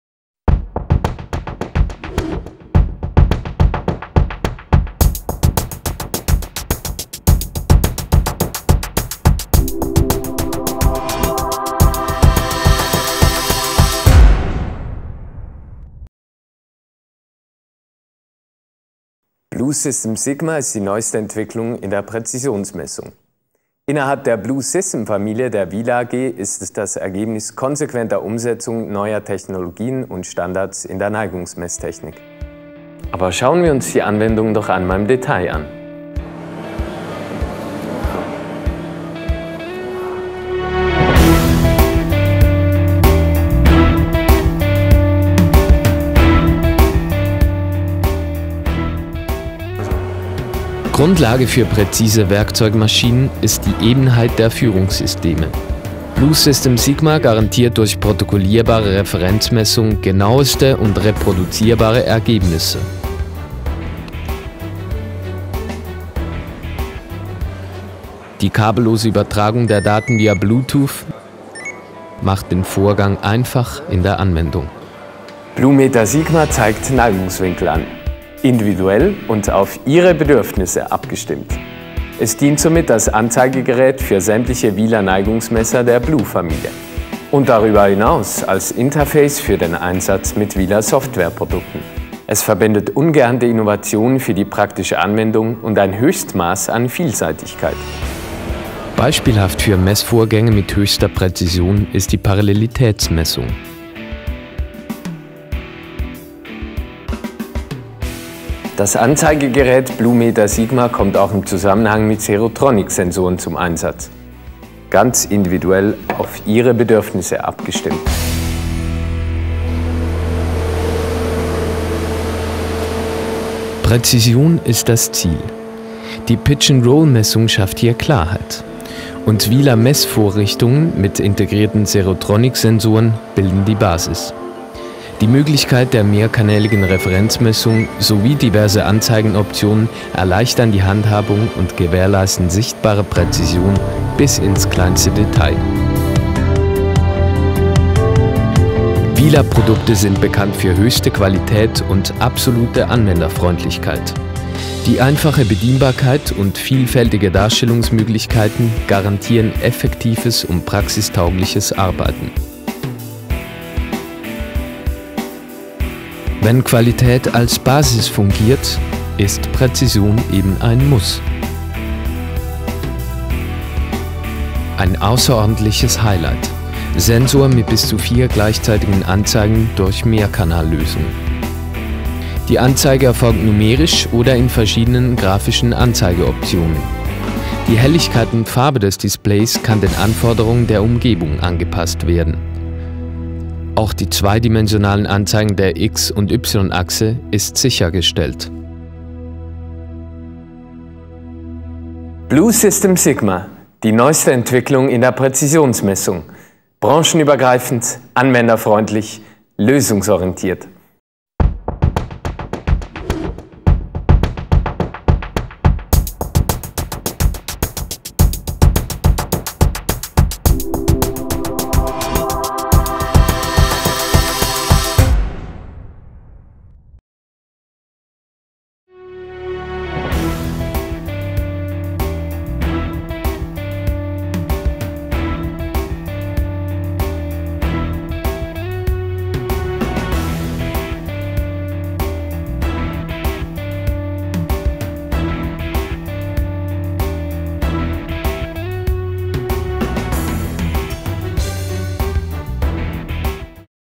BlueSYSTEM SIGMA · Praezisionsneigungsmessgerät der WYLER AG
Die neueste Generation von hochpräzisen elektronischen Neigungsmessern, bzw. Neigungsmessgeräten der BlueSYSTEM-Familie sind speziell für die Präzisionsmessung kleinster Neigungen geeignet. Dazu gehören insbesondere die Ebenheitsmessung von Mess- und Kontrollplatten oder die Vermessung der Geometrieeigenschaften an Maschinen aller Art.

Das neue BlueSYSTEM ist eine konsequente Weiterentwicklung der bekannten und bewährten Neigungsmessgeräte, Neigungsmesser MINILEVEL NT und LEVELTRONIC NT. Ein BlueSYSTEM besteht normalerweise aus einem oder zwei Messgeräten BlueLEVEL und einem Anzeigegerät BlueMETER SIGMA. Je nach Anwendung ist das BlueMETER SIGMA zusätzlich mit einem PC mit entsprechender Software verbunden, mit welcher die Messwerte online ausgewertet und dargestellt werden können.